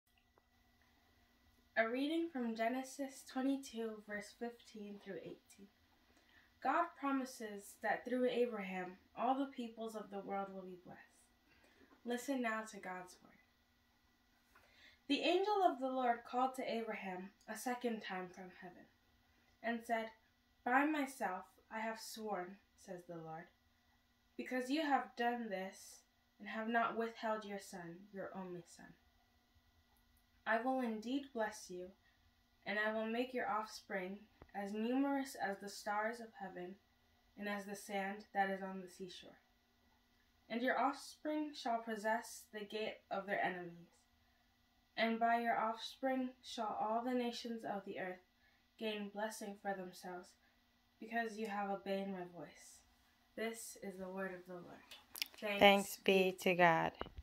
The 2nd Lesson - Genesis 22:15-18 - Read by Regina G.
The Second Lesson - God promises that, through Abraham, all the peoples of the world will be blessed

Genesis 22:15-18

Liturgist:  Regina G.